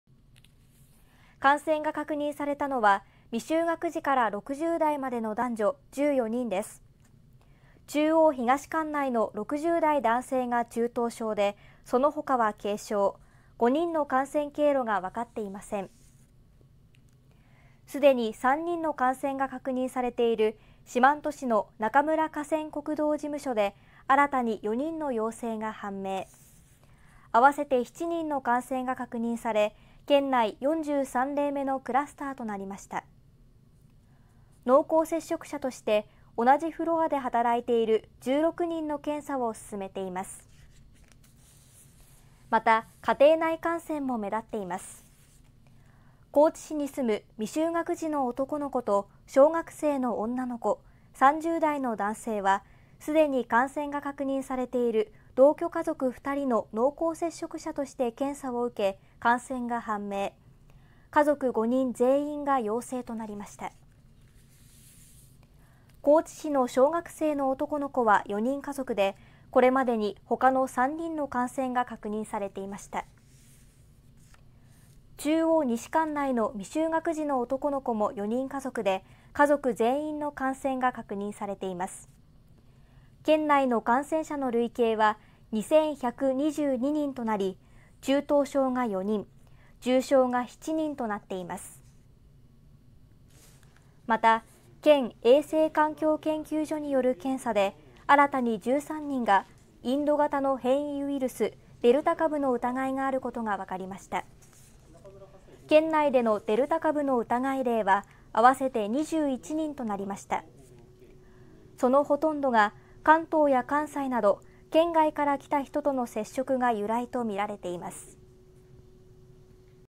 高知県で新たに14人の感染確認 職場クラスターが発生 【高知】 (21/08/03 18:50)
高知県で新たに14人が新型コロナウイルスに感染していることが分かりました。四万十市で職場クラスターが発生、家庭内感染も広がっています。

感染が確認されたのは未就学児から60代までの男女14人です。中央東管内の60代男性が中等症で、そのほかは軽症、5人の感染経路が分かっていません。

すでに3人の感染が確認されている四万十市の中村河川国道事務所で新たに4人の陽性が判明。合わせて7人の感染が確認され、県内43例目のクラスターとなりました。濃厚接触者として同じフロアで働いている16人の検査を進めています。

また、家庭内感染も目立っています。高知市に住む未就学児の男の子と小学生の女の子、30代の男性はすでに感染が確認されている同居家族2人の濃厚接触者として検査を受け感染が判明。家族5人全員が陽性となりました。

高知市の小学生の男の子は4人家族で、これまでにほかの3人の感染が確認されていました。

中央西管内の未就学児の男の子も4人家族で家族全員の感染が確認されています。

県内の感染者の累計は2122人となり中等症が4人、重症が7人となっています。

また、県衛生環境研究所による検査で新たに13人がインド型の変異ウイルス・デルタ株の疑いがあることが分かりました。県内でのデルタ株の疑い例は合わせて21人となりました。そのほとんどが関東や関西など県外から来た人との接触が由来とみられています。